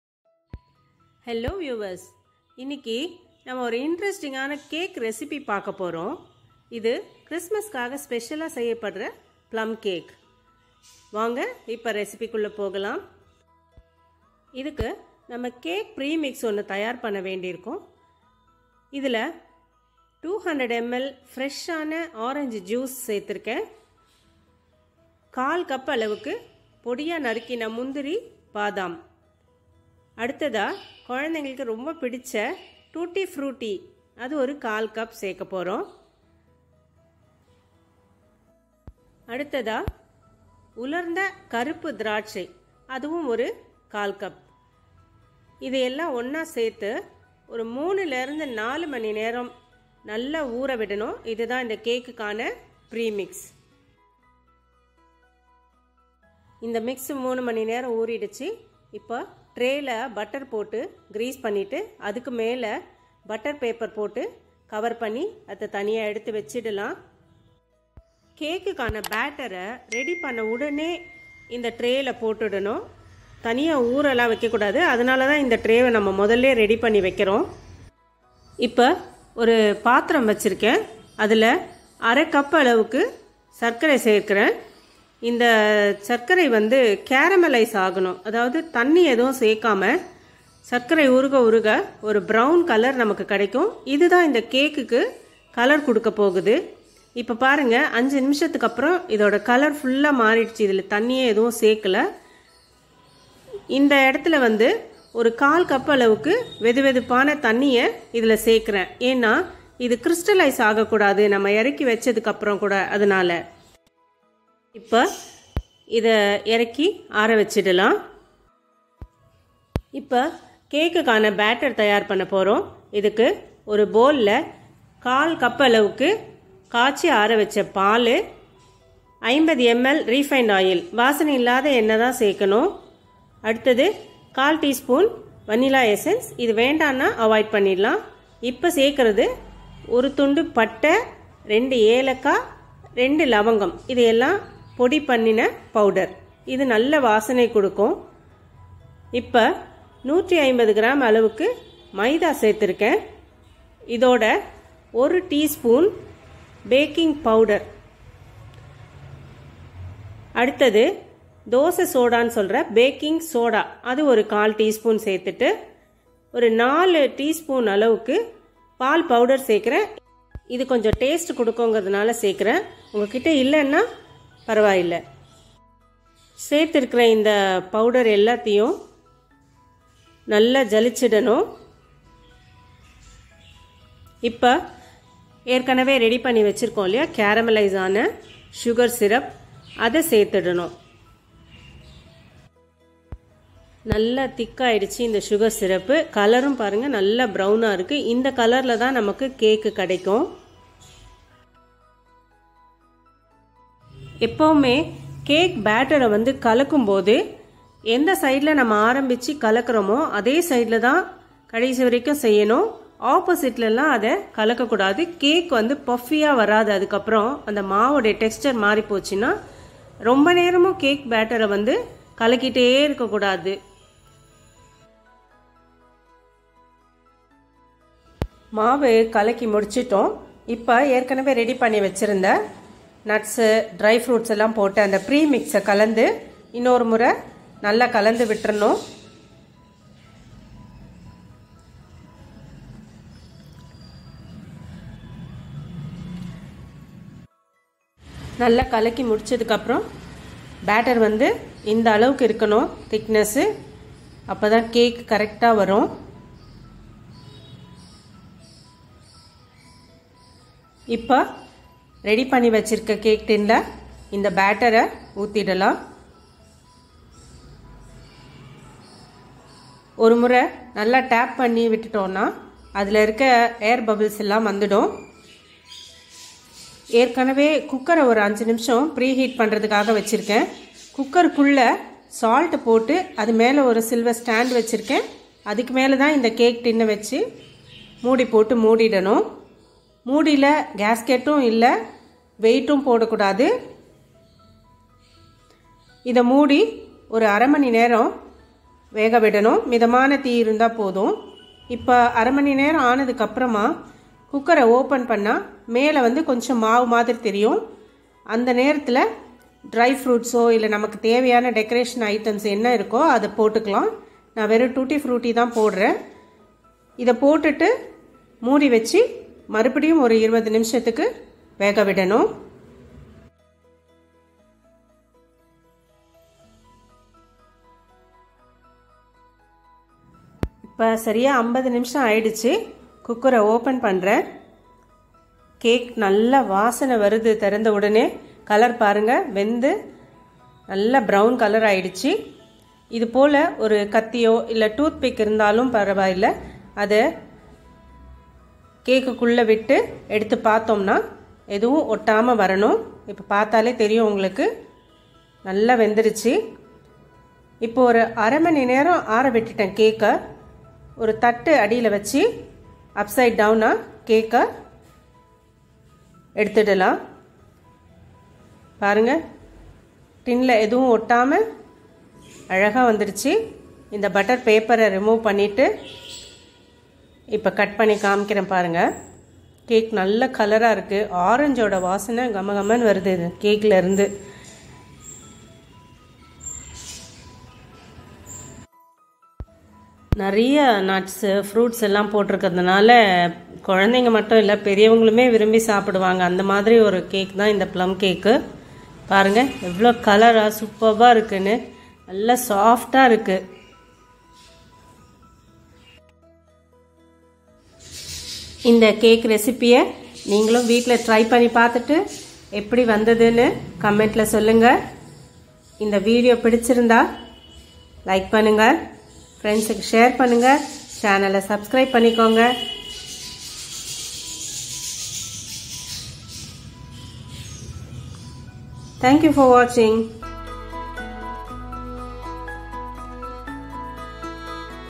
no bake cooker cake/no egg/oven/curd/Plum cake/ ப்ளம்கேக்/கிறிஸ்துமஸ் கேக்
plumcack/ப்ளம்கேக்
ஆரஞ்ச்ஜுஸ்-200ml
மைதா-150g
சர்க்கரை1/2கப்
டூட்டி-ப்ரூட்டி1/4 கப்
முந்திரி1/4கப்

உலர்ந்த கருப்புதிராட்சை1/4கப்
பட்டை1துண்டு
லவங்கம்2
ஏலக்காய்2
50mlரீபைண்ட்ஆயில்
1ஸ்பூன் பேக்கிங் பவுுடர்
1/2ஸ்பூன் பேக்கிங் சோடா
4ஸ்பூன் பால் பவுடர்.